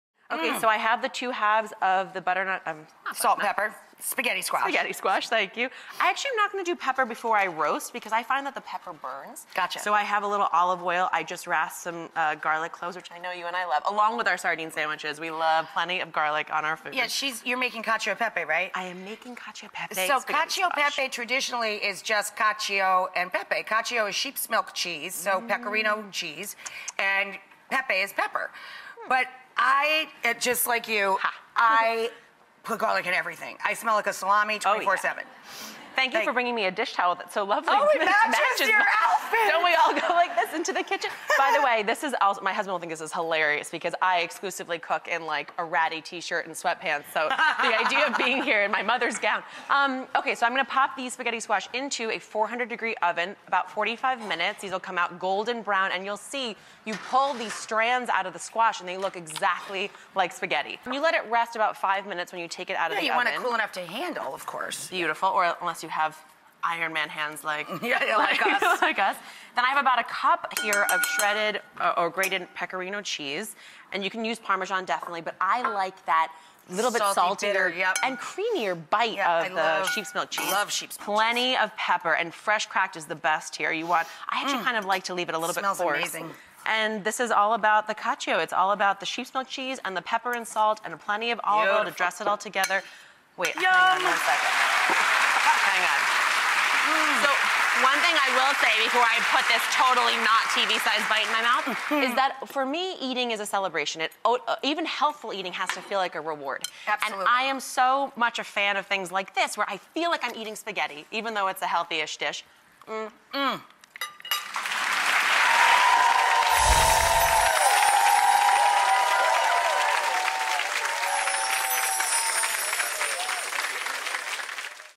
Daphne Oz's Lightened-Up Cacio E Pepe with Spaghetti Squash
Daphne Oz, author of The Happy Cook, pairs the Italian duo of "cheese and pepper" with spaghetti squash for a filling and deceptively indulgent dinner.